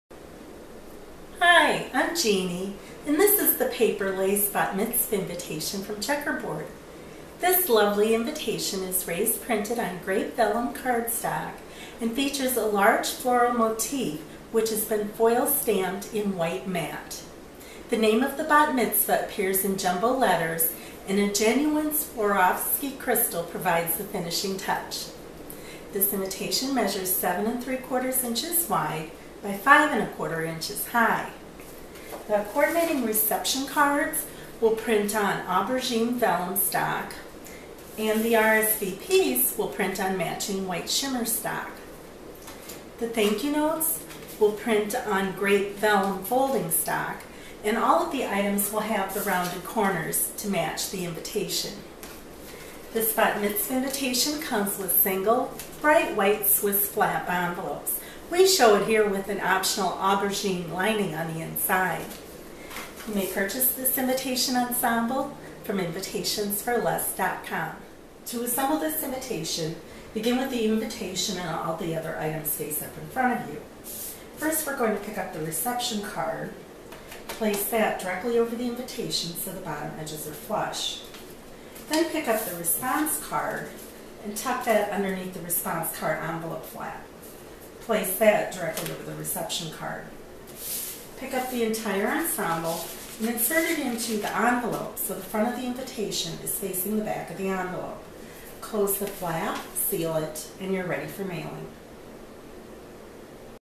Bat Mitzvah Invitation - Paper Lace
Demonstration and Assembly Instructions for the Paper Lace Bat Mitzvah Invitation from Invitations4Less.com.